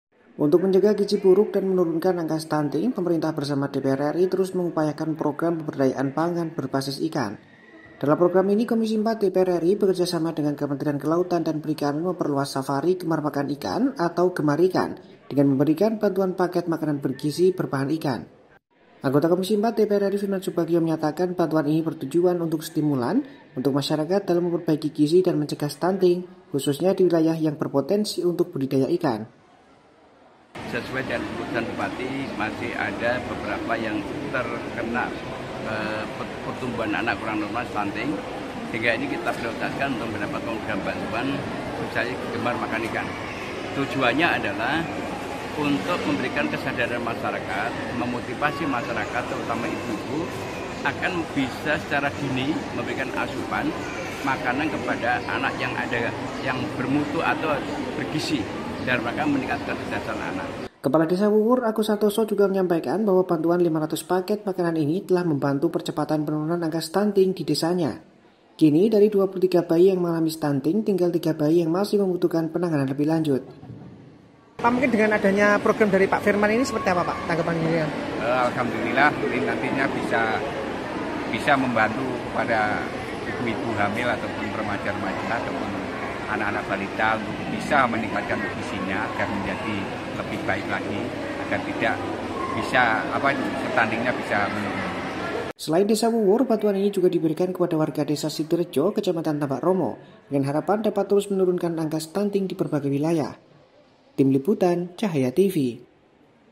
Diharapkan Program Gemar Makan Ikan Jadi Solusi Cegah Gizi Buruk Dan Stunting
Pemerintah dan dpr ri bekerja sama dalam mencegah gizi buruk dan menurunkan angka stunting melalui program gemar makan ikan  bantuan yang di berikan berupa bantuan paket makanan bergizi berbahan ikan di berbagai daerah
Untuk mencegah gizi buruk dan menurunkan angka stunting, pemerintah bersama dpr ri terus mengupayakan program pemberdayaan pangan berbasis ikandalam program ini, komisi iv dpr ri bekerja sama dengan kementerian kelautan dan perikanan memperluas safari "gemar makan ikan" atau gemarikan, dengan memberikan bantuan paket makanan bergizi berbahan ikan